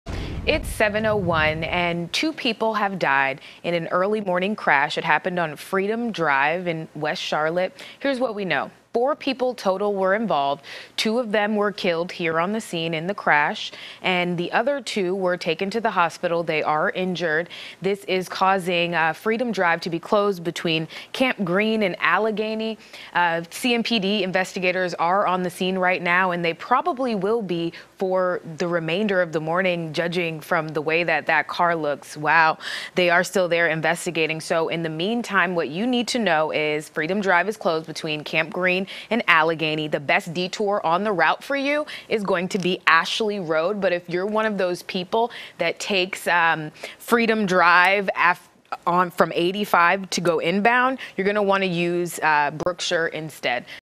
2 killed, 2 injured in crash on Freedom Drive in west Charlotte
Two people are dead after a crash on Freedom Drive in west Charlotte Tuesday morning, officials confirm.